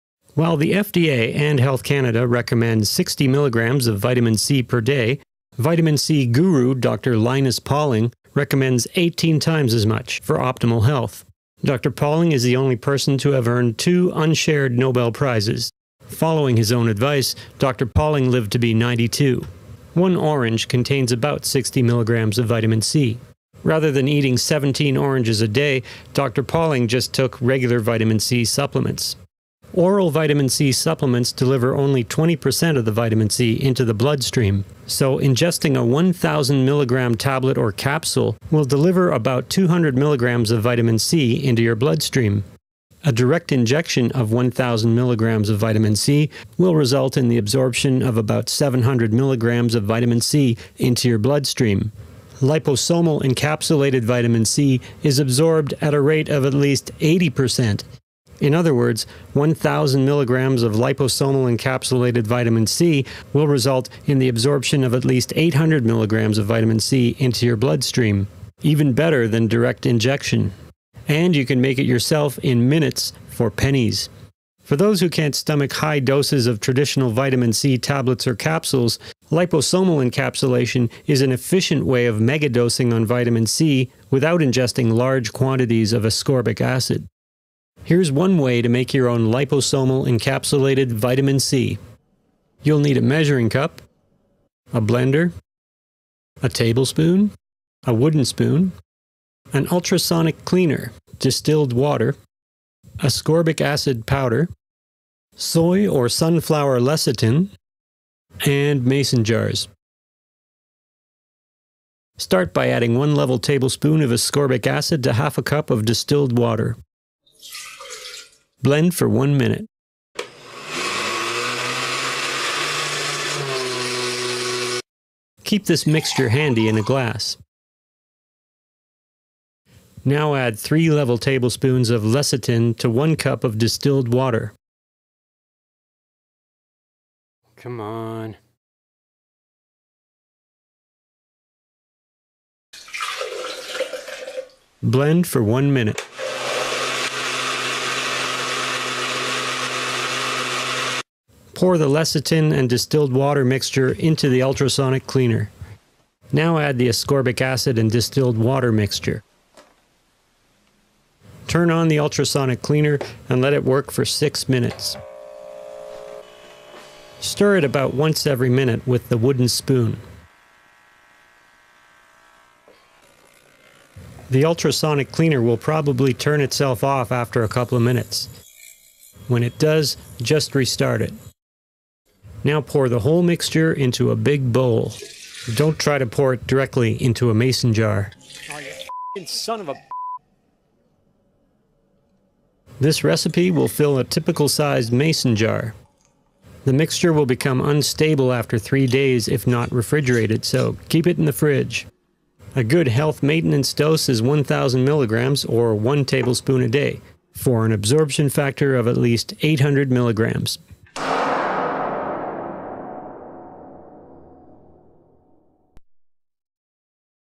*Liposomales Vitamin C*Herstellen*Mega Dosis*Energie Booster*
Hallo meine lieben Freunde ;-)Das ist ein Video für eure Gesundheit
70 Menschen auf meinem Kanal das freut mich und motiviert mich noch mehr für Euch alle da draußen zu machen.
Herzlichen Dank Euer Robin Hood ;-)